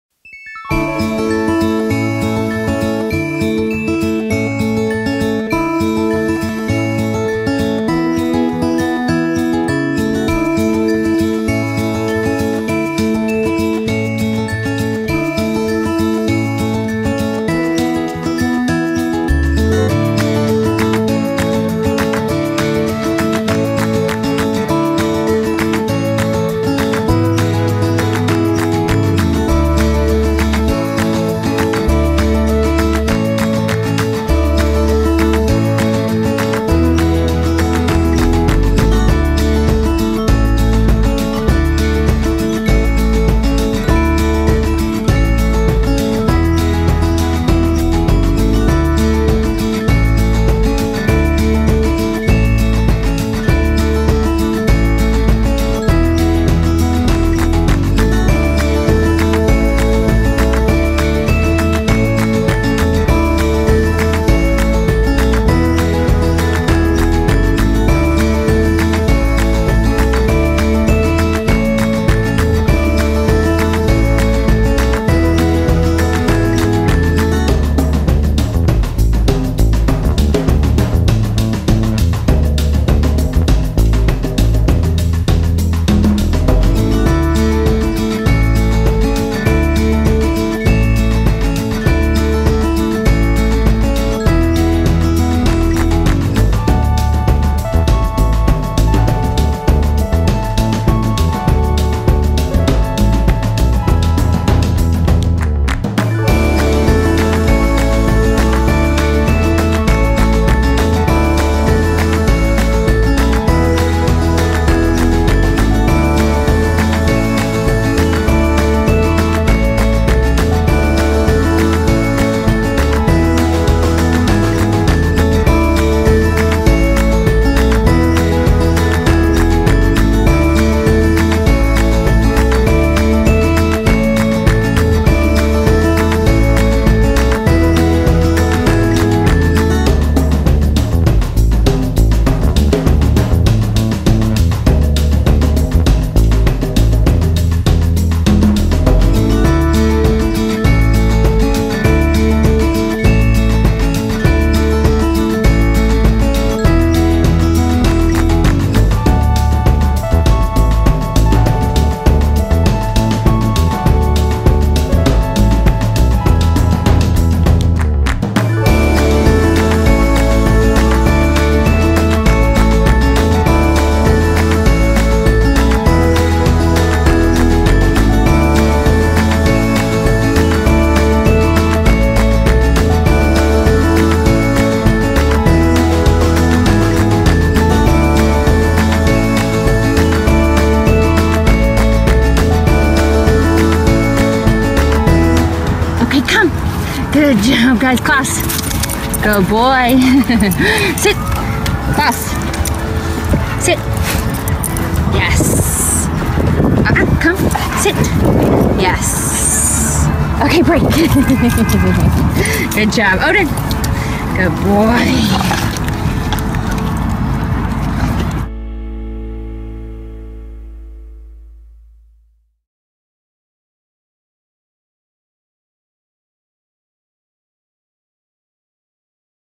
Klaus & Odin: Training Field Trips
Klaus and Odin's training field trips to Old Town Alexandria and the National Mall.